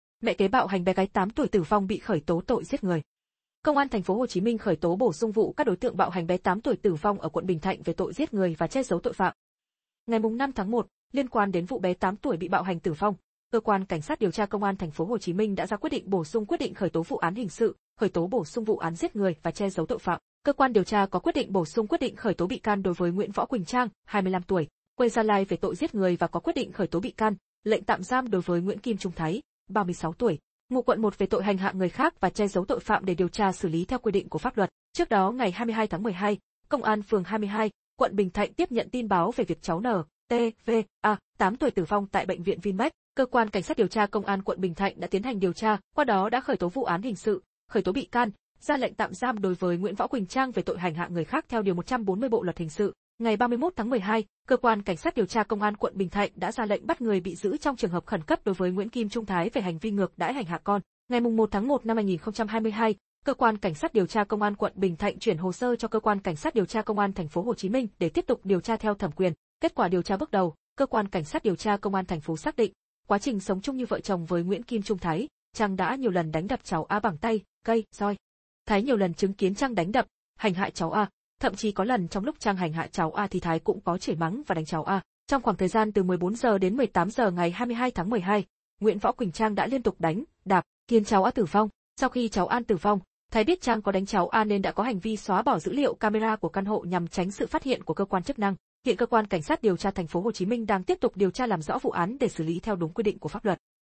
"Mẹ kế" bạo hành bé gái 8 tuổi tử vong bị khởi tố tội giết người
Công an TPHCM khởi tố bổ sung các đối tượng bạo hành bé 8 tuổi tử vong ở quận Bình Thạnh về tội Giết người và Che giấu tội phạm.

Ngày 5/1, liên quan đến vụ bé 8 tuổi bị bạo hành tử vong, Cơ quan Cảnh sát điều tra Công an TPHCM đã ra quyết định bổ sung quyết định khởi tố vụ án hình sự, khởi tố bổ sung vụ án "Giết người" và "Che giấu tội phạm".